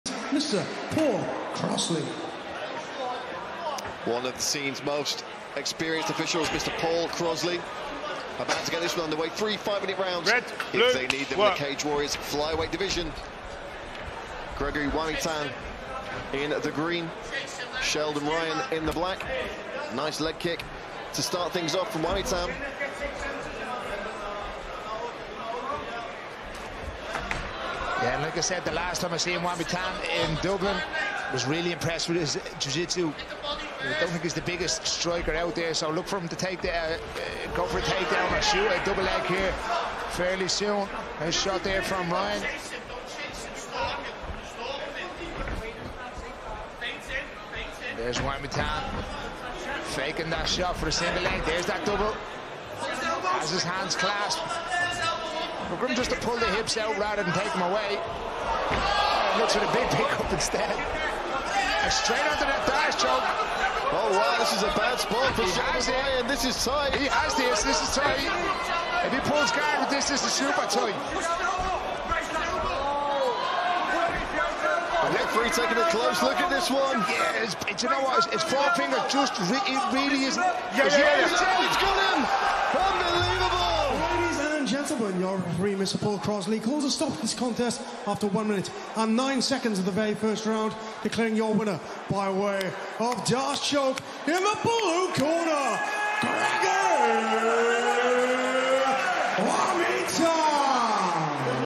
CRAZY D’ARCE CHOKE | Sheldon Ryan VS. Gregory Wamytan | FULL FIGHT | CW 178 Manchester
Full pro prelim bout between Sheldon Ryan VS. Gregory Wamytan at cw178 Manchester!